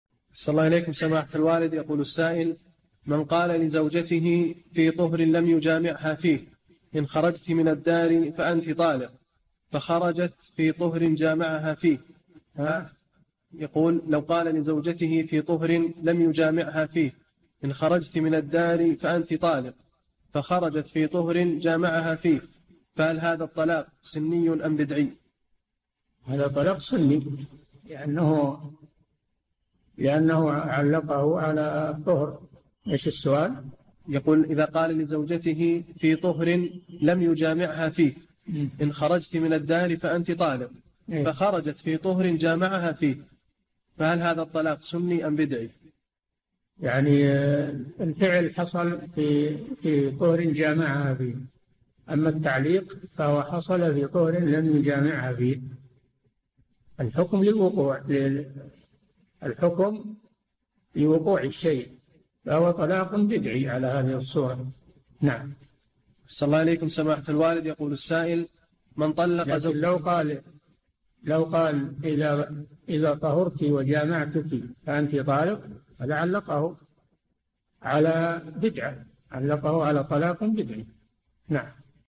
[2212- 4717] قال لزوجته في طهر لم يجامعها فيه: إن خرجت من الدار فأنت طالق، فخرجت في طهر جامعها فيه!
[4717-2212] من قال لزوجته في طهر لم يجامعها فيه: إن خرجتِ من الدار فأنتِ طالق، فخرجت في طهر جامعها فيه فهل هذا الطلاق سني أم بدعي؟
الجَوَابُ: يعني الفعل حصل في طهر جامعها فيه، أما التعليق فهو حصل في طهر لم يجامعها فيه، الحكم للوقوع، الحكم لوقوع شيء فهو طلاق بدعي على هذه الصورة. لكن لو قال إذا طهرتِ وجامعتك فأنتِ طالق فهذا علقه على بدعة، علقه على طلاق بدعي.
السيرة الذاتية :
للاشتراك في قناة الشيخ صالح الفوزان
لمشاهدة حلقات فتاوى على الهواء كاملة

للاستفسار والإقتراحات والدعم
رئيس الفريق
لمشروع كبار العلماء